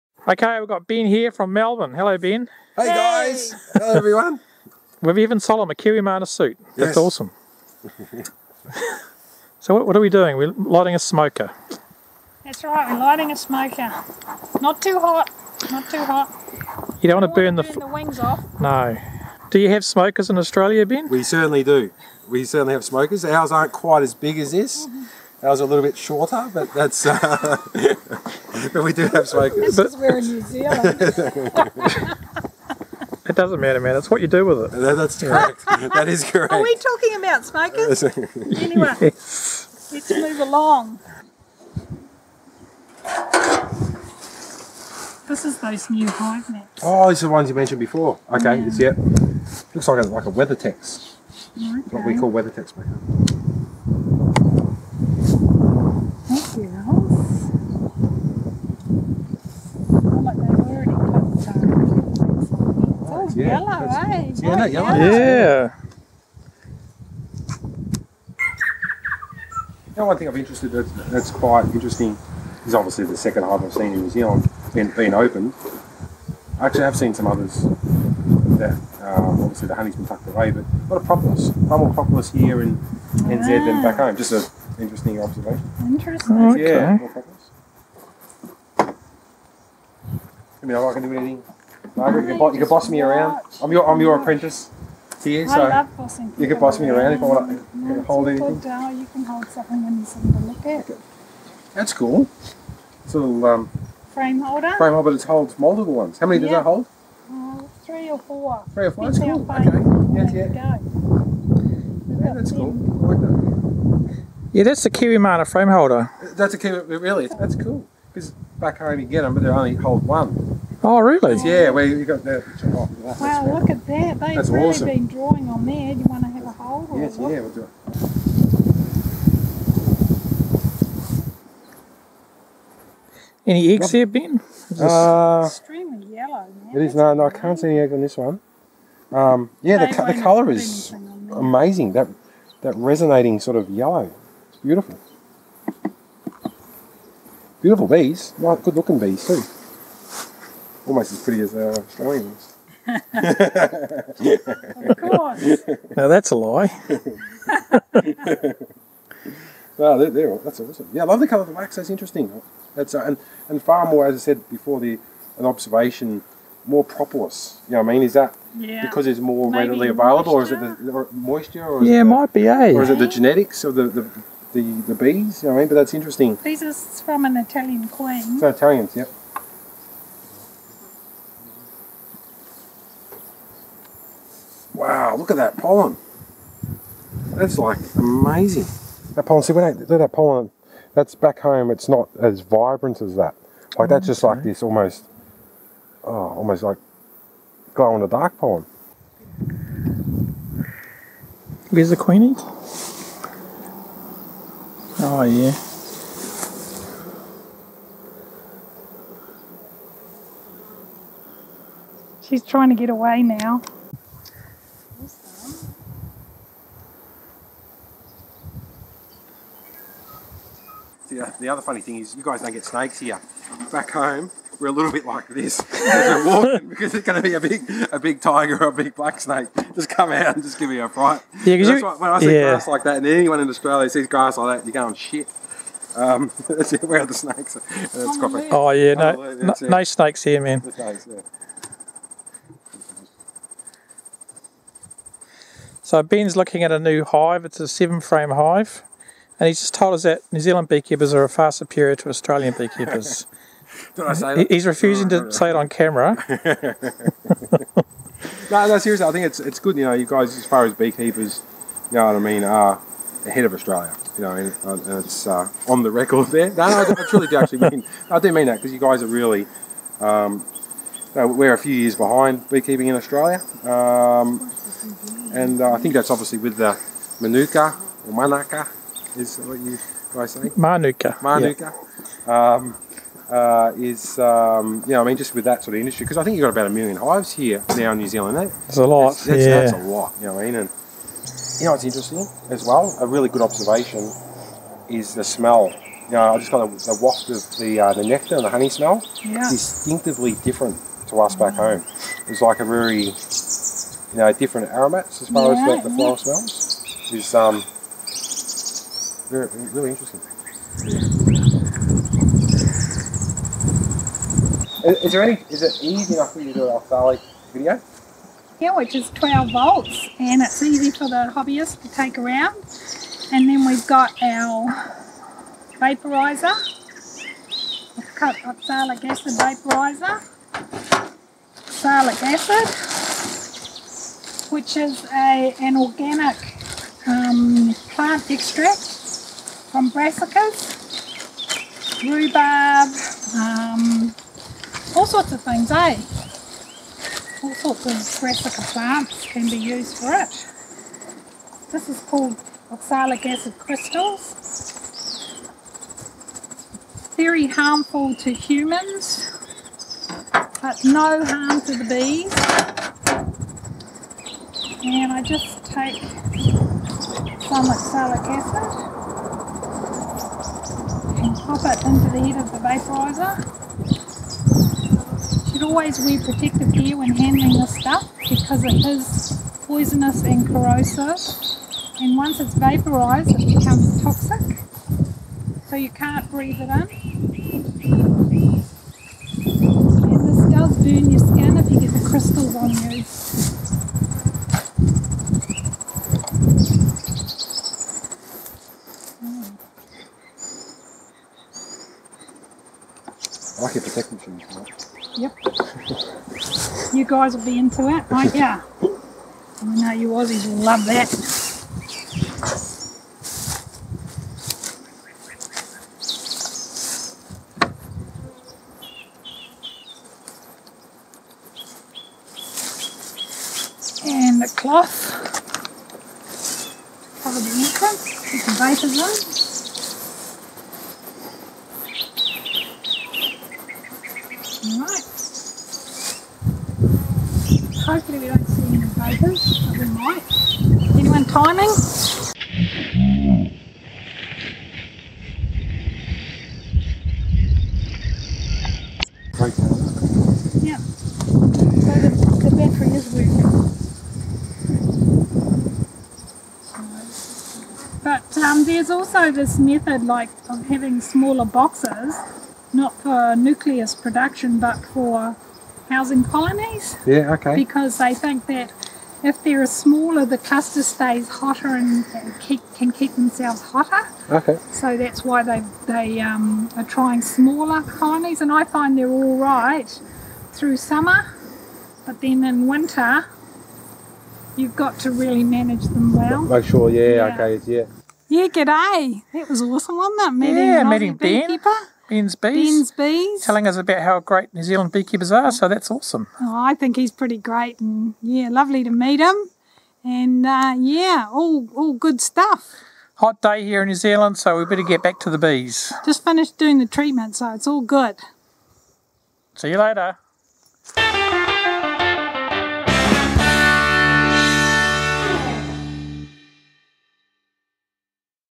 A Visitor from Australia - We meet Ben from Ben's Bees
Ben Moore was visiting from Australia, he visited us at kiwimana HQ and tells us how Beekeepers in New Zealand are far superior to the ones in Australia and that we are much better at playing Rugby.

We also show Ben how we treat for Varroa mites, something that Australia currently do not have.